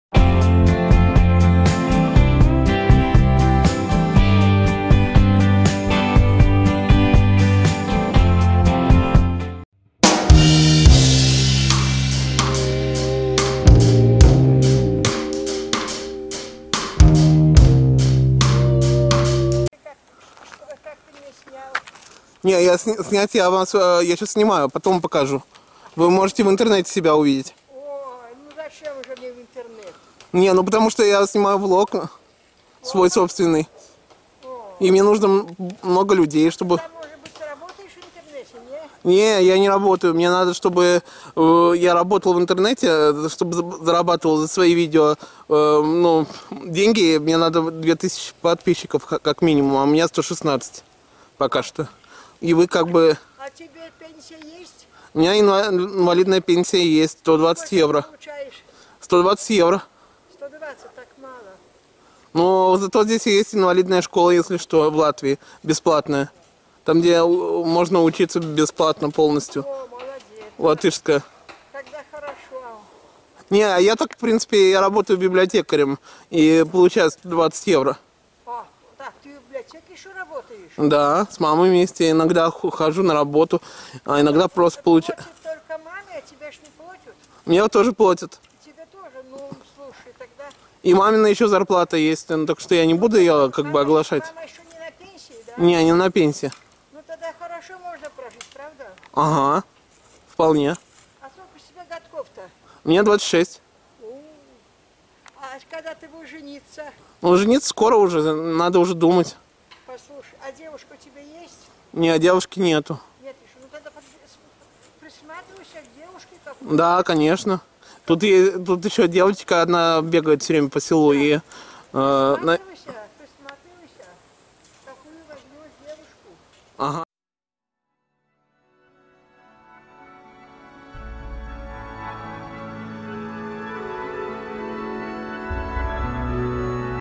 76. Интервью с бабушкой Людой
Это видео покажет на сколько комуникабельный человек.